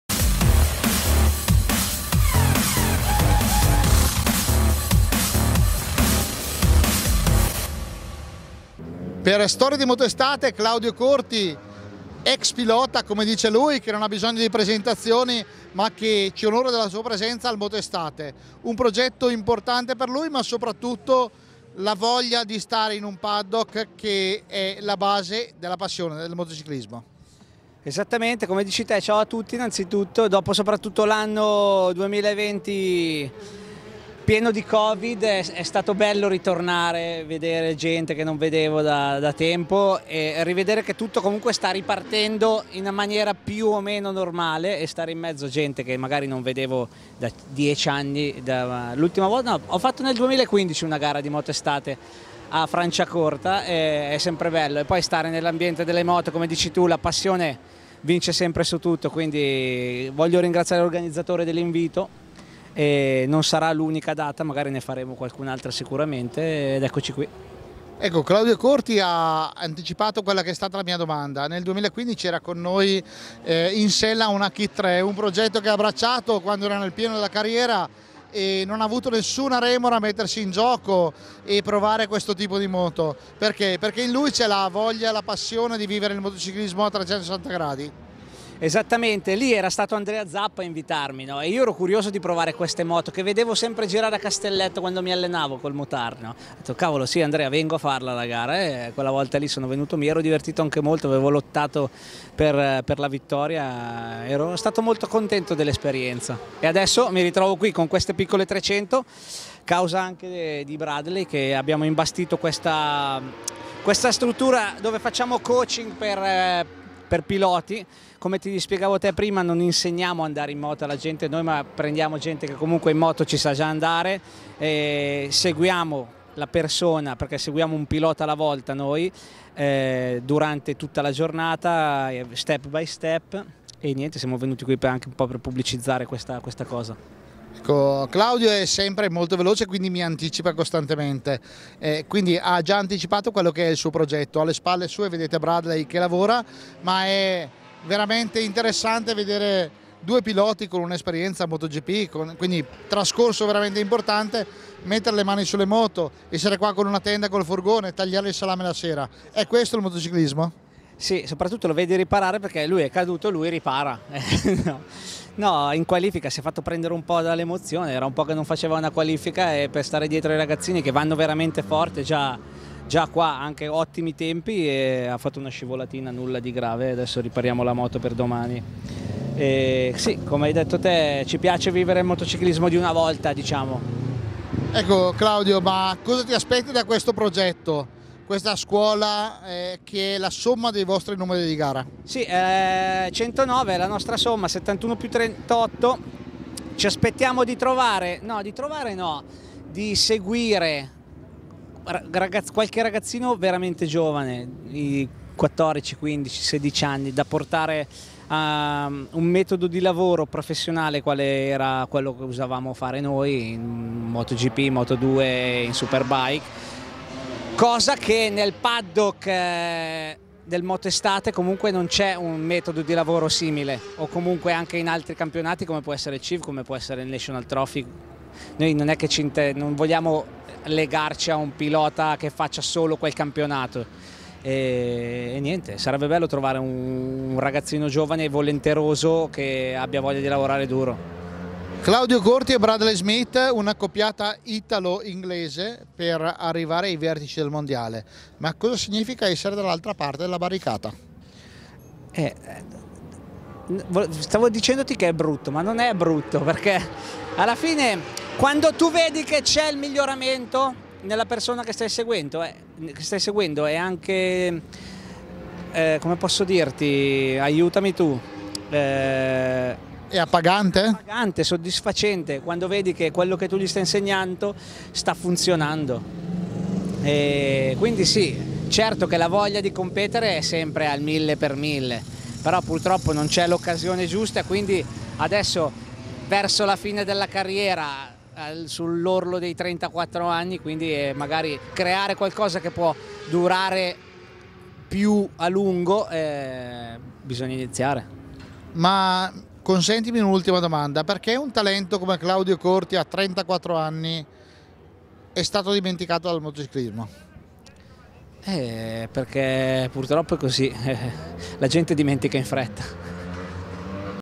"STORIE DI MOTOESTATE" L'EX PILOTA MOTOGP CLAUDIO CORTI SI RACCONTA.
In questa puntata il pilota italiano ex MotoGP Claudio Corti parla del suo nuovo progetto di coaching #109, del suo passato da motociclista e del suo futuro.